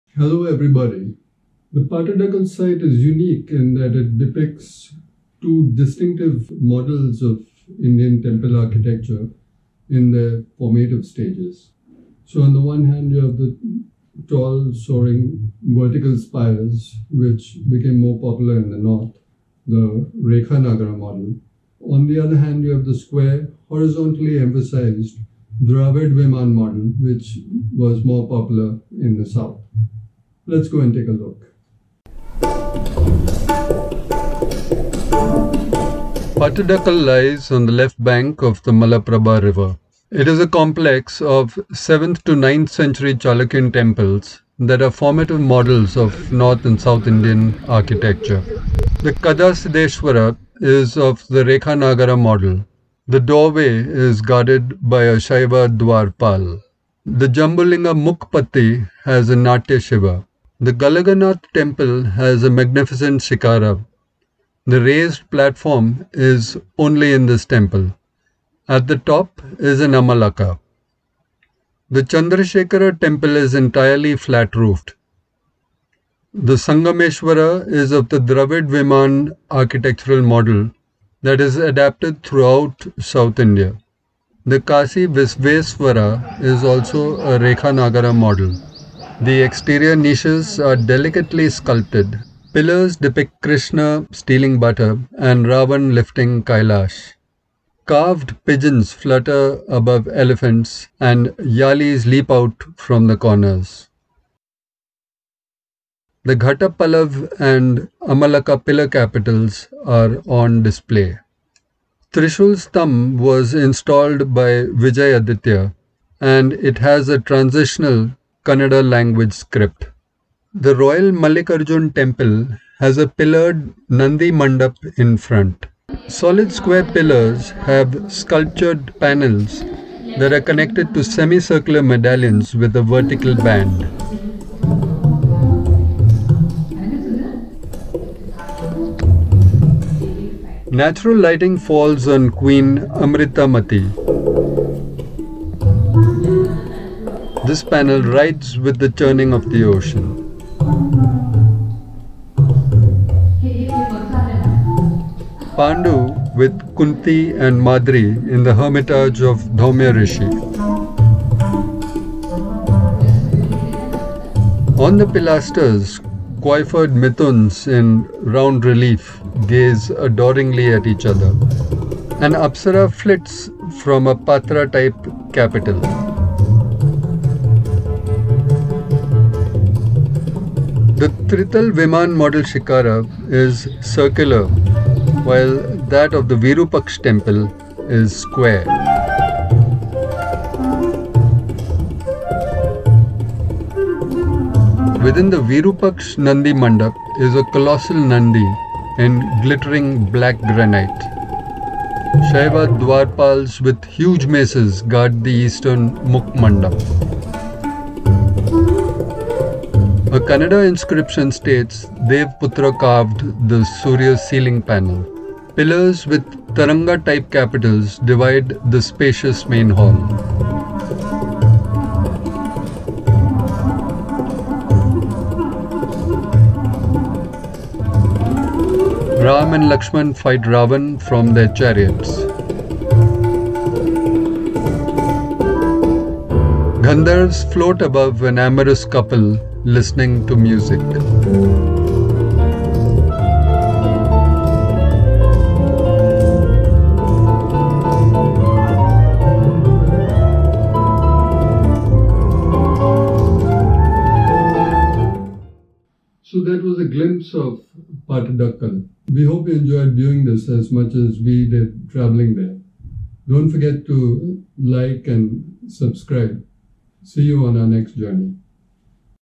Pattadakal | Early Chalukya Temple Architecture | el Liven
Pattadakal has sublime temple architecture evolving from basic stone structures to intricate exterior and interior work. This UNESCO site depicts the rekha-nagara and dravida-viman models in their formative stages.

Description:
Pattadakal lies on the left bank of the Malaprabha river. It is a complex of 10 early Chalukyan temples that are formative models of north and south Indian architecture.
The Kadasiddhesvara is of the rekha-nagara model  The doorway is ornate and guarded by Saiva dvara-pala.  The Jambulinga mukha-patti is a panel of natya Siva.  The Galagantha temple has a magnificent sikhara.  The raised platform is (found) only in this temple. At the top is an amalaka.  The Chandrashekara temple is entirely flat roofed.  The Sangamesvara  is of the dravida viman architectural model that is adapted through the entire south India.
The Kasi Visvesvara is of the rekha-nagara model.  The exterior niches are delicately scultpted  and perforated to the interior.  Pillars depict Krishna stealing butter, and  Ravan lifting Kailasa. In it carved pigeons flutter above the beam holding elephants, and  yalis leap out from the corners. The Ghata-pallava and amalaka pillar capitals are on display. The Trisul stambha was installed by Vijayaditya and has a transitional Kannada language script.
The royal Mallikarjun temple has a pillared Nandi mandapa in front. Solid square pillars without a base form a central nave The double crossed corbels are of the mushti and taranga types. Semicircular medallions  with a vertical band are carved into  intricate floral and other designs. Sculptured panels are in the middle zone.  Natural lighting falls on queen Amritamati. While this panel writhes with the churning of the ocean.  Pandu with Kunti and Madri in the hermitage of Dhaumya rishi.  On the pilasters coiffured mithunas in round relief gaze adoringly at each other.  An apsara flits from a patra type capital.  The round tritala-viman model sikhara is circular while that of the fully evolved virupaksh temple is square.
Within its Nandi-mandapa is a colossal Nandi in glittering black granite.  Saiva dvara-palas with huge maces guard the eastern mukh-mandapa. A Kannada inscription states Devaputra carved the Surya ceiling panel.  Pillars with Taranga type capitals divide the spacious main hall.  Ram and lakshman fight ravana from their chariots.  An amorous couple is seated on a couch listening to music  while gandharvas float above them.

Archaeological Survey of India. World Heritage Series. Pattadakal. Government of India. 2008

Chapters:
0:00 Intro
0:45 Early temples
1:00 Developed models
1:23 Kasivisesvara
2:00 Mallikarjuna
3:05 Virupaksh